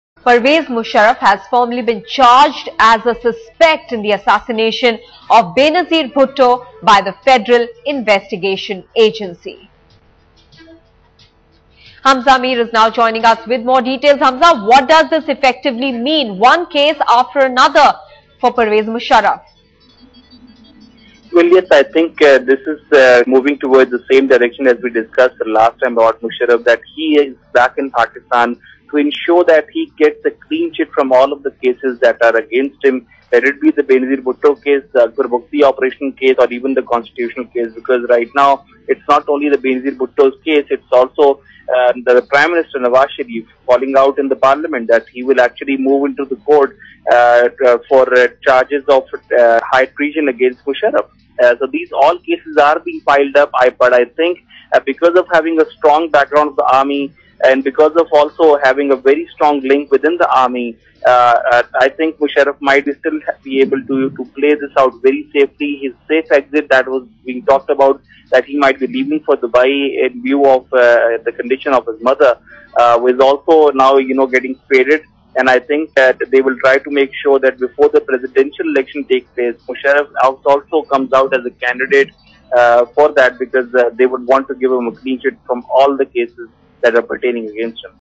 Musharraf officially charged as suspect in Bhutto assassination
Besides being charged in the Benazir Bhutto murder case, former Pakistan president, Pervez Musharraf, could also be put on trial for high treason.